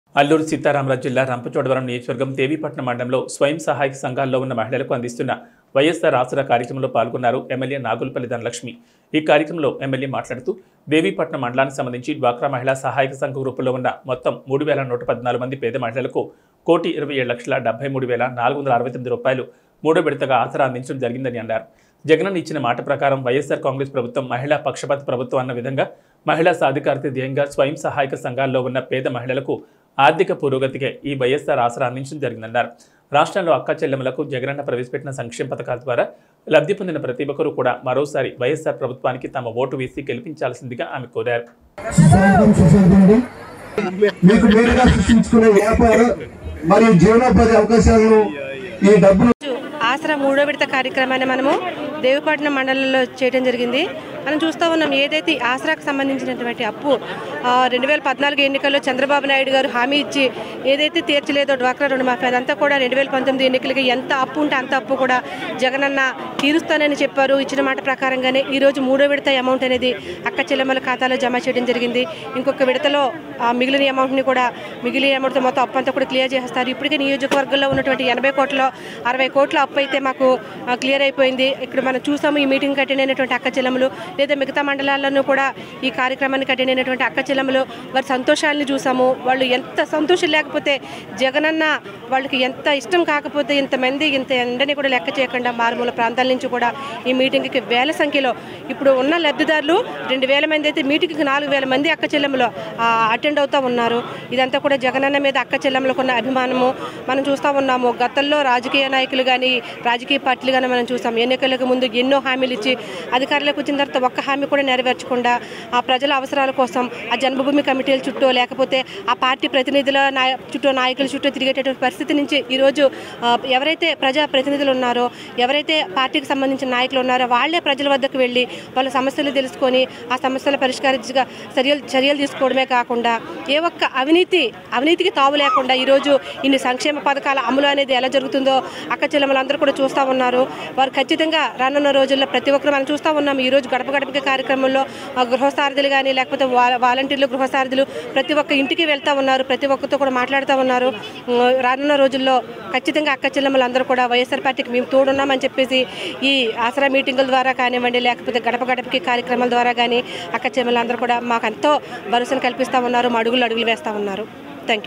రంపచోడవరం||వైఎస్సార్ ఆసరా కార్యక్రమానికి ముఖ్య అతిథిగా ఎమ్మెల్యే ధనలక్ష్మి
@truthnewsteluguరంపచోడవరం||వైఎస్సార్ ఆసరా కార్యక్రమానికి ముఖ్య అతిథిగా ఎమ్మెల్యే ధనలక్ష్మి